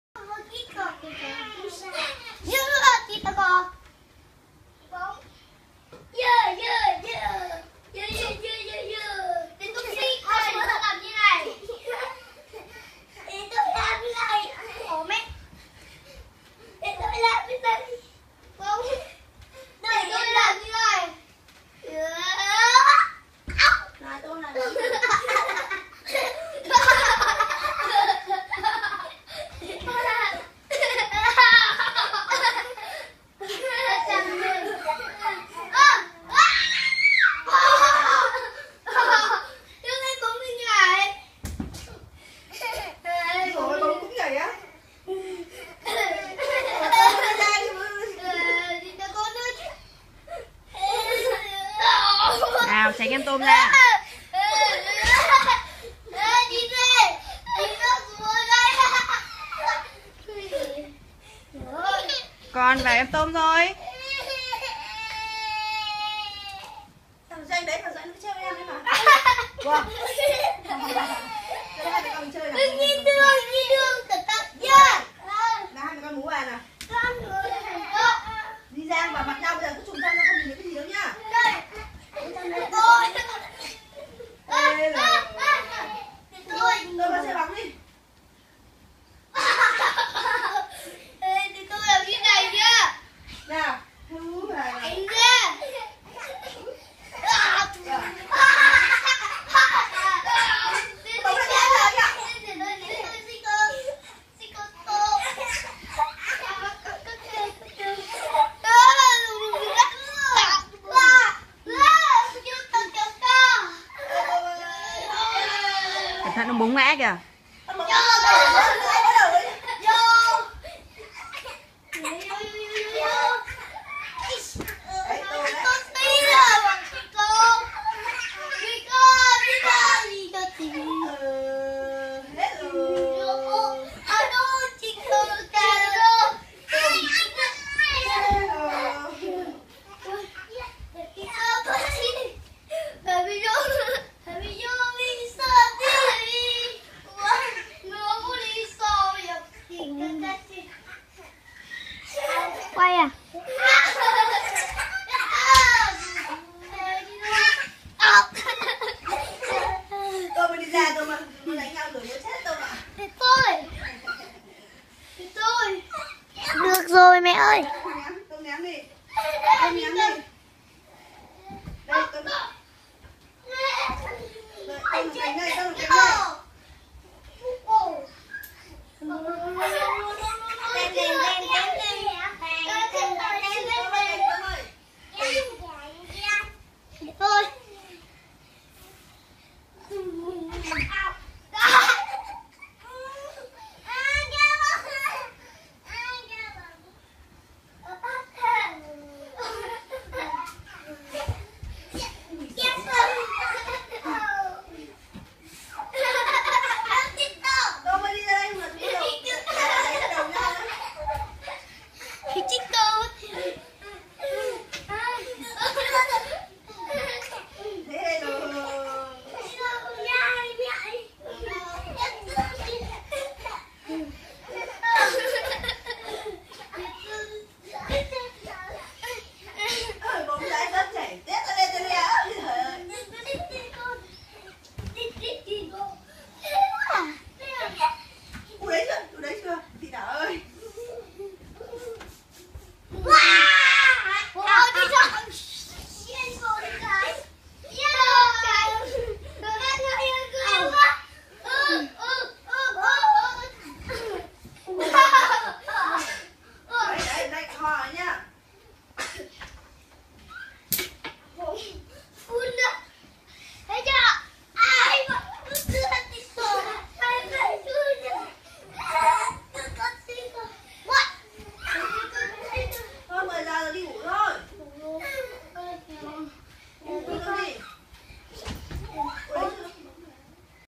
Tít Mít đại náo cùng đồng bọn. Tit Mit has a tantrum with his accomplices
Cuộc sống hàng ngày của anh em sinh đôi có gì khác. Hãy cùng xem nhé.
The daily life of the twins is very special. Let's see together